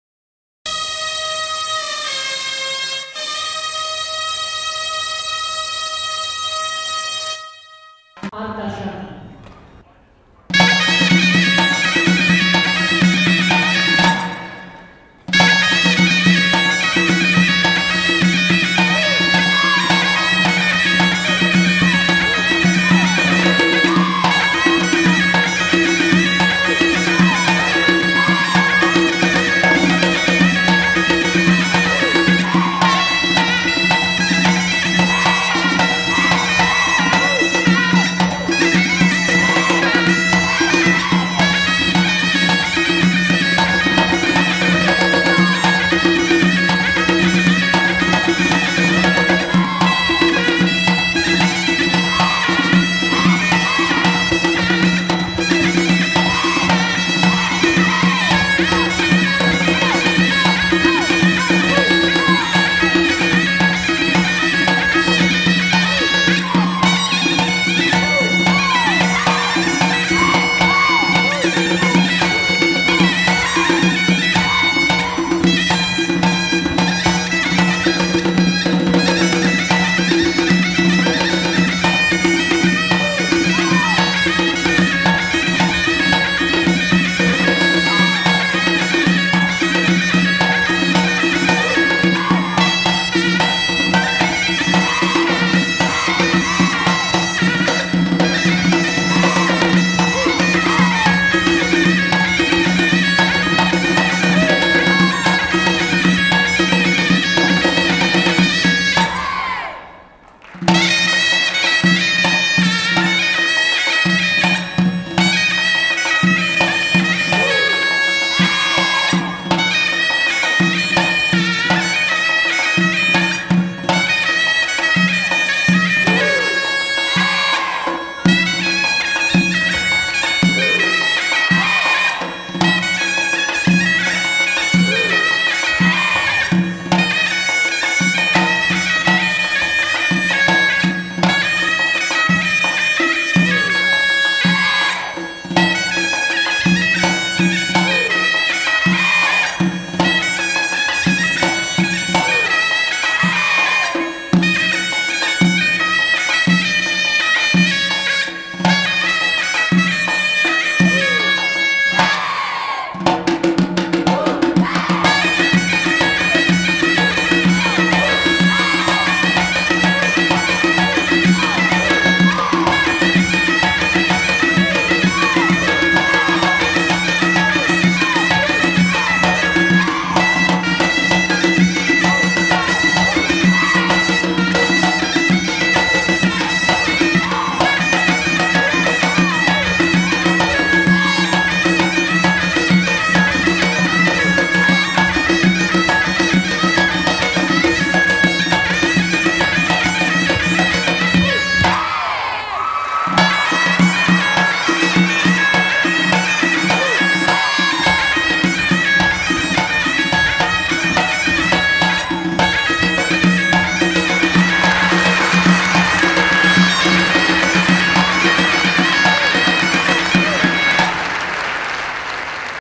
Vanoush Khanamerian Dance School - Artashati Par - Armenian Traditional Dance
Artashati dance performed by Vanoush Khanamerian Dance School, honoring Vanoush Khanamerian's 75th birthday in 2003.